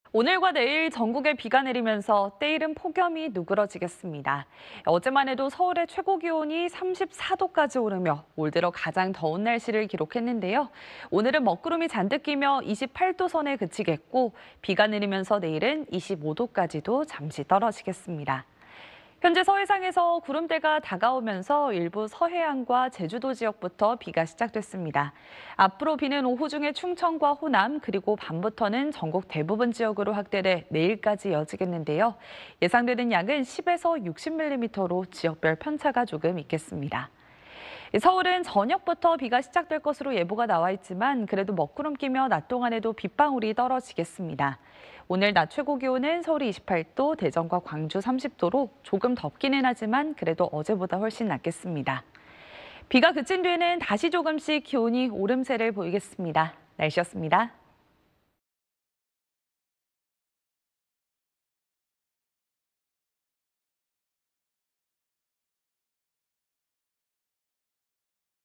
[날씨] 전국에 비 내리며 폭염 누그러져…서울 낮 28도 / SBS
오늘(20일)과 내일 전국에 비가 내리면서 때 이른 폭염이 누그러지겠습니다.

어제만 해도 서울의 최고기온이 34도까지 오르며 올 들어 가장 더운 날씨를 기록했는데요, 오늘은 먹구름이 잔뜩 끼며 28도선에 그치겠고 비가 내리면서 내일은 25도까지도 잠시 떨어지겠습니다.

현재 서해상에서 구름대가 다가오면서 일부 서해안과 제주도 지역부터 비가 시작됐습니다.

앞으로 비는 오후 중에 충청과 호남, 그리고 밤부터는 전국 대부분 지역으로 확대돼 내일까지 이어지겠는데요, 예상되는 양은 10~60mm로 지역별 편차가 조금 있겠습니다.

서울은 저녁부터 비가 시작될 것으로 예보가 나와 있지만 그래도 먹구름 끼며 낮 동안에도 빗방울이 떨어지겠습니다.

오늘 낮 최고 기온은 서울이 28도, 대전과 광주 30도로 조금 덥기는 하지만 그래도 어제보다 훨씬 낫겠습니다.

비가 그친 뒤에는 다시 조금씩 기온이 오름세를 보이겠습니다.

(양태빈 기상캐스터)